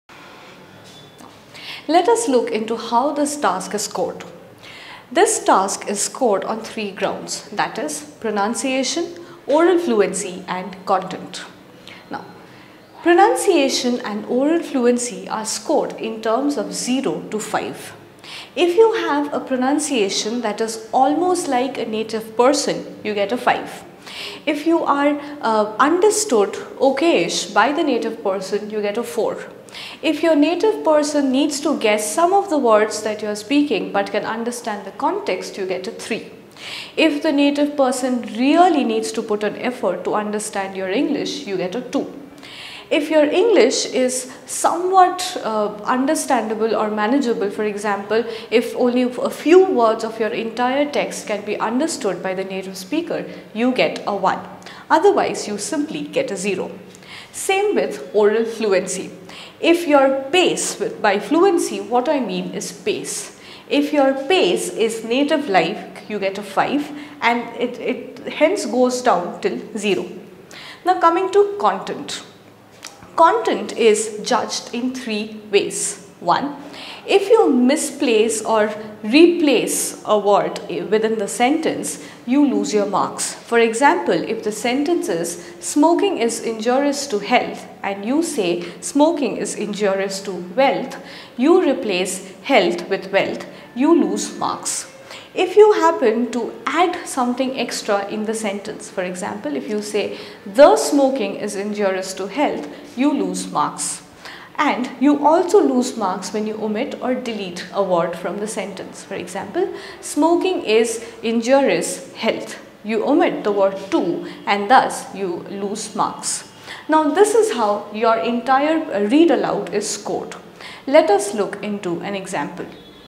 PTE Speaking Read Aloud Scoring
Let us look into how this task is scored.this task is scored on 3 grounds
i.e
1. Pronounciation
2. Oral fluency
3. Content
pronounciation and oral fluency are scored in terms of 0 to 5
if you have a pronounciation that is almost like a native person u get a 5
if you are understood ok by the native person u get a 4
if a native person needs to guess somw of the words ie speaking but understand u get  a 3
if a native person really needs to put a effort to understand yuour english you get a 2
if your english some what understandable and managable for example if few words of your entire text can be
 undertood  by the speaker you get a 1
otherwise you simply get a zero
same with oral fluency if your base by fluency
if your pace is native like you get 5 and hence goes down to zero
now coming to content ,content is judged in 3 ways
1.if you misplace or replace a word in the sentence you will lose your marks
for ex. smoking is injurious to health and you say smoking is injurious to wealth you replace health
with wealth if you happen to add something extra in the sentence
for eg: the smoking is injurious to health you loose marks and you also lose marks with omit and delete
from the sentence :for eg: smoking is injurious health . you lose your marks